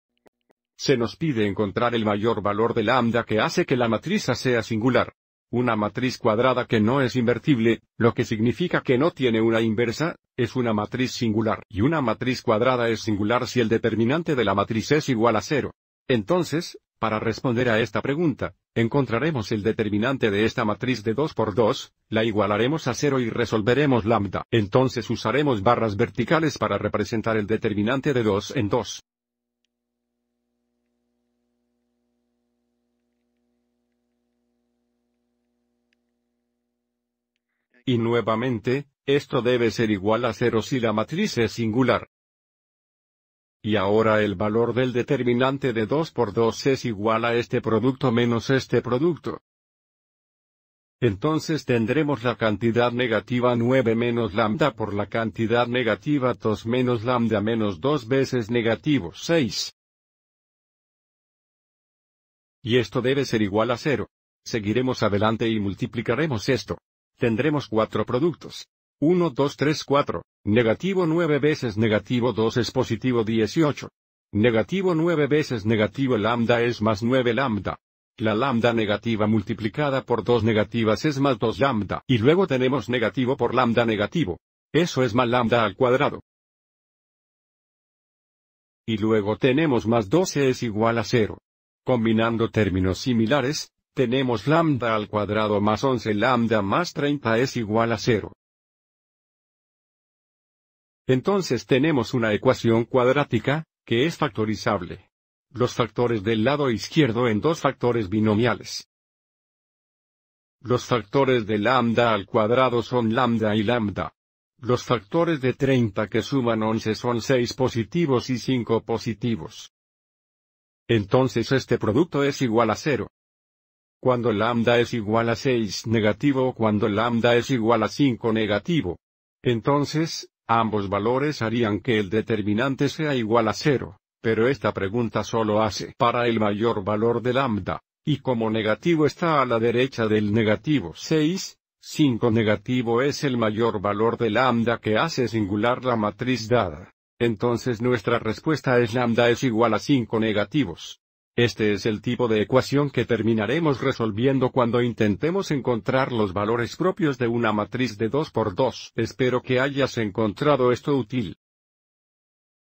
Ej .: Encuentre un valor para hacer una matriz singular de 2x2 (calentamiento de valor propio)
Este video explica cómo determinar los valores de lambda que hacen que una matriz de 2x2 sea singular.